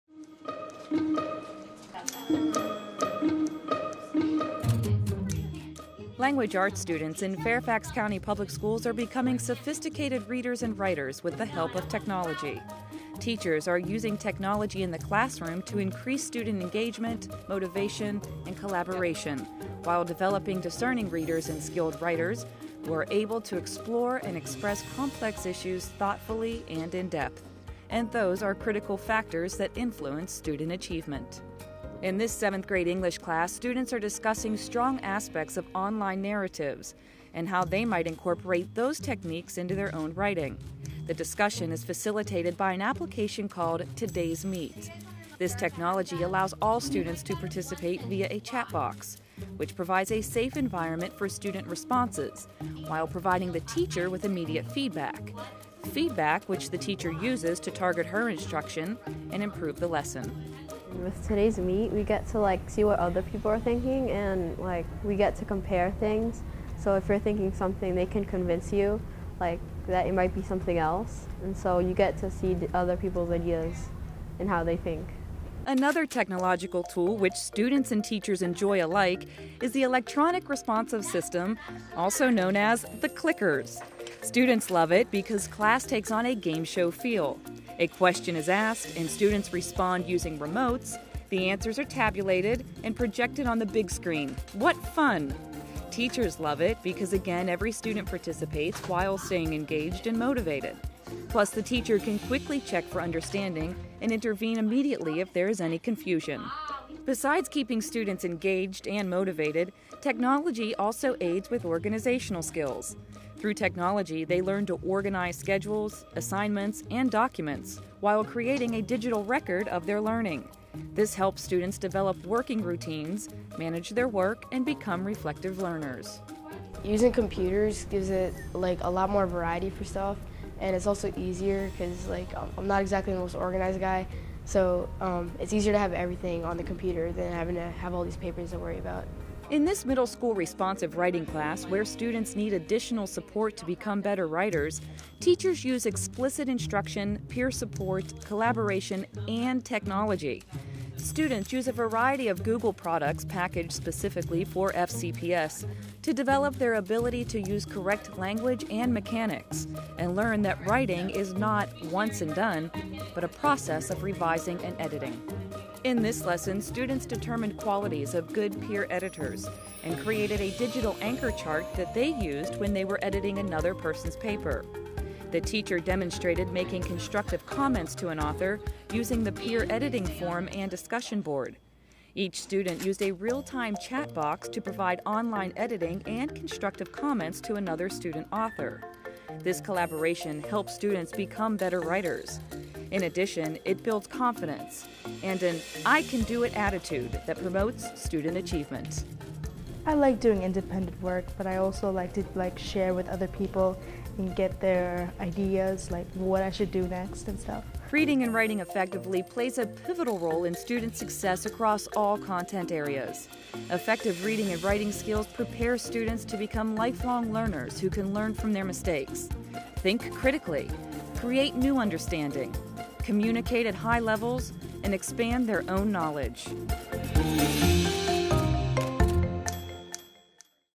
SAG Video - Language Arts
Fairfax County Language Arts teachers use technology to improve lesson plans and encourage their students to express themselves.